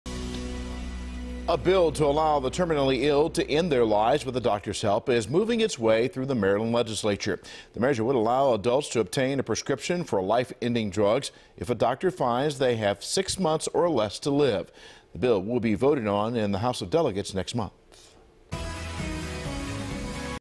Measure Allowing Life-Ending Drugs Advances To Md. House Of Delegates
Two Maryland legislative panels have advanced a bill to allow the terminally ill to end their lives with a doctor's help.